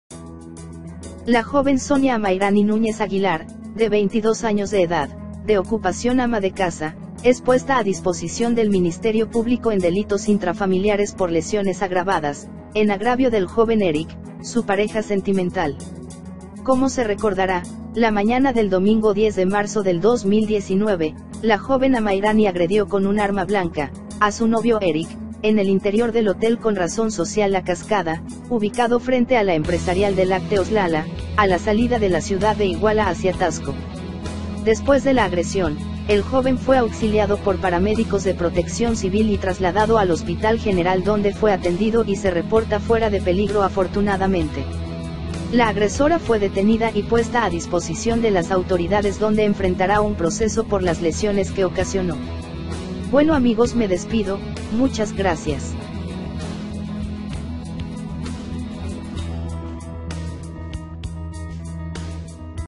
QUIEN ES La Novia Que APUÑAL0 a Su Novio En MOTEL La Cascada de IGUALA Amayrani Núñez
Amayrani Núñez reacciono de manera inapropiada al interior de la habitación número 16 del hotel “La Cascada” que se ubica en la ciudad Industrial de Iguala, .